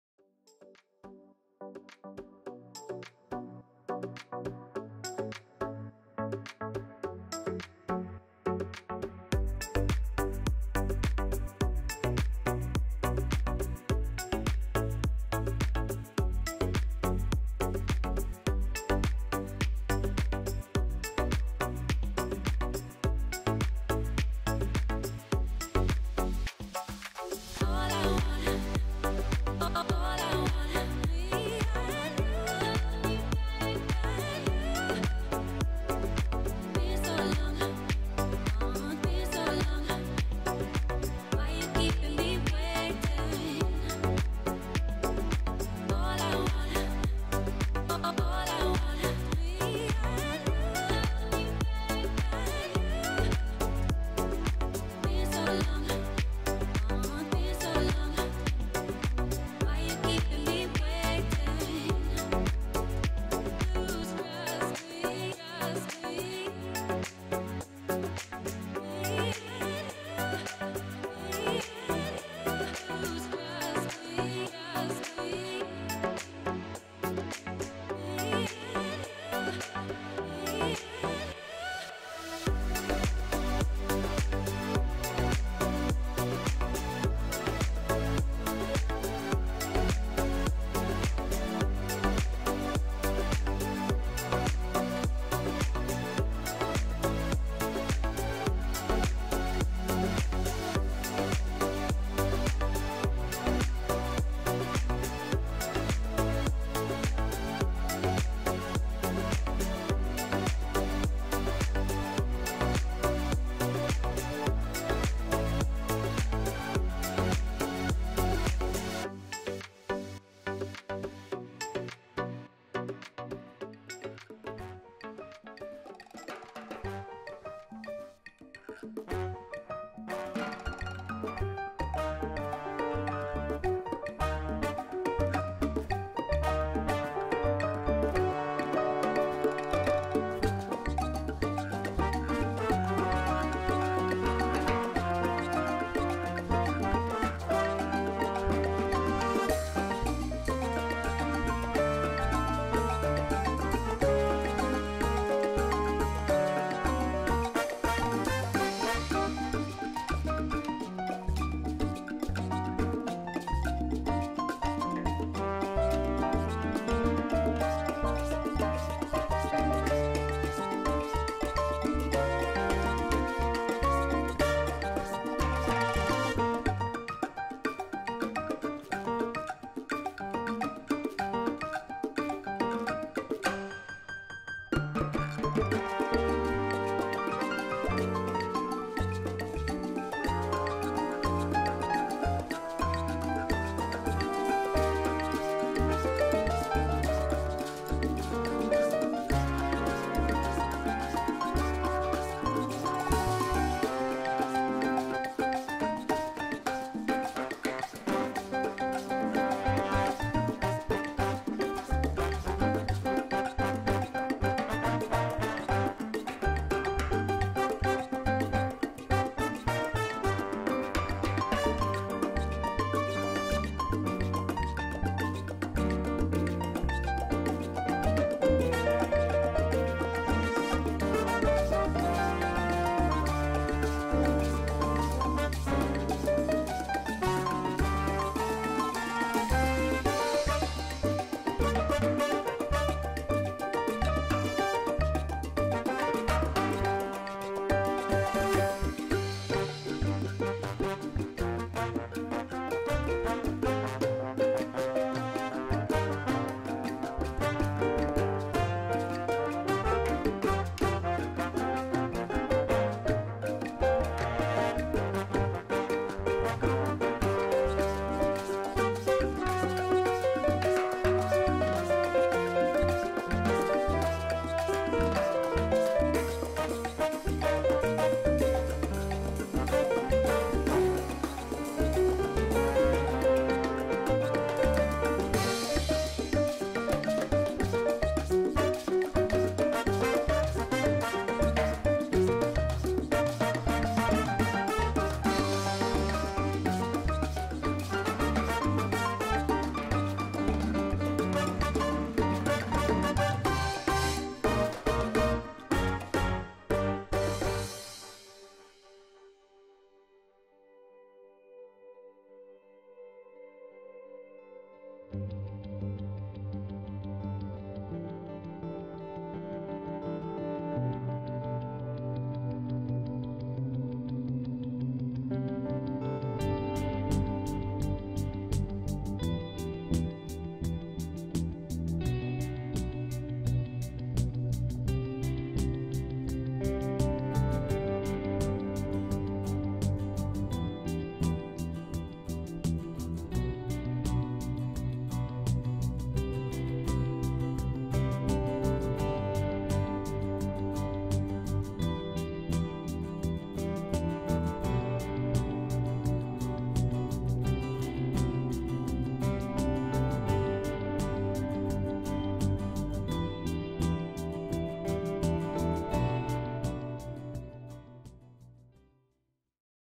Playa Escondida, Fajardo, Puerto Rico; Slow TV
A 4K drone flight along Playa Escondida (the "hidden beach") and Playa Colora (the "colour beach") in Fajardo, Puerto Rico. Whilst tempting, swimming is not recommended here due to strong, dangerous currents.